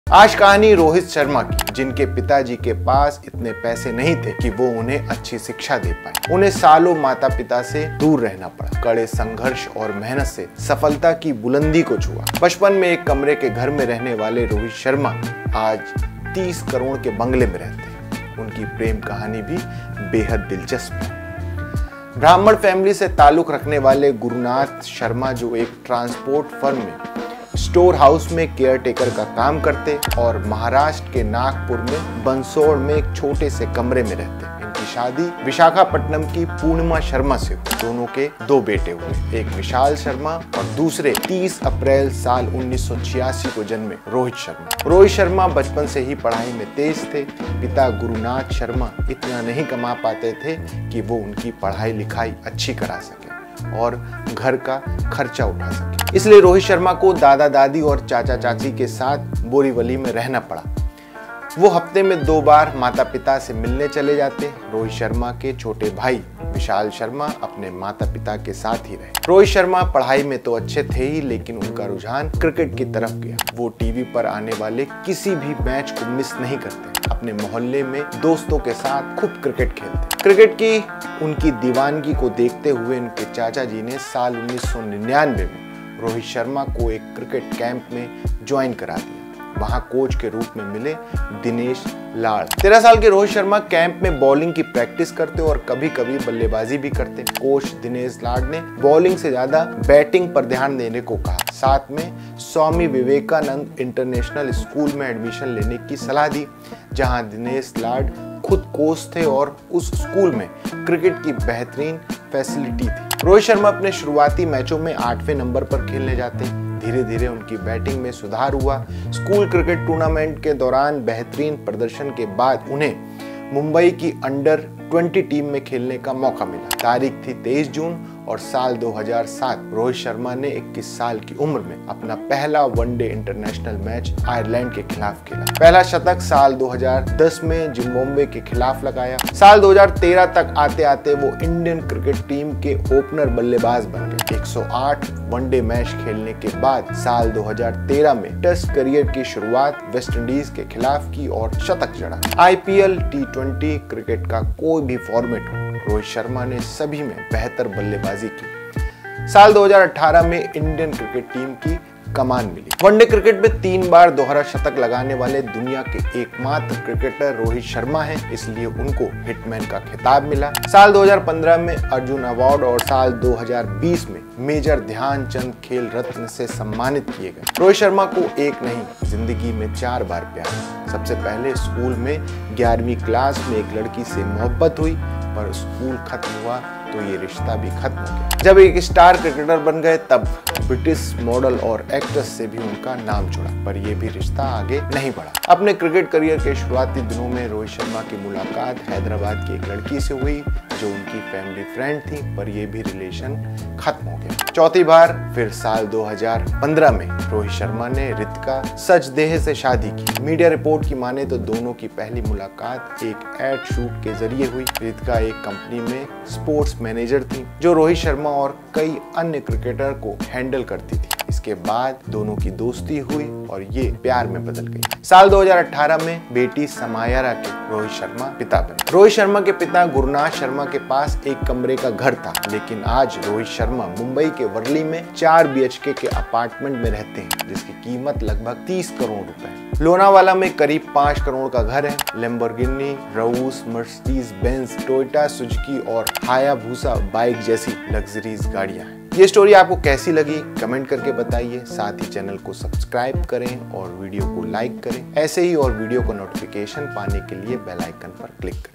Cricketer Rohit Sharma की Story, जब पिता के पास फीस भरने तक पैसे नहीं थे।
रोहित शर्मा (Rohit Sharma) के पिताजी के पास इतने पैसे नहीं थे वो उन्हें अच्छी शिक्षा दे पाएं। उन्हें सालों माता-पिता से सालों दूर रहना पड़ा। कड़े संघर्ष और मेहनत से सफलता की बुलंदी को छुआ। बचपन में एक कमरे के घर में रहने वाले रोहित शर्मा आज 30 करोड़ के बंगले में रहते हैं। उनकी प्रेम कहानी भी बेहद दिलचस्प है।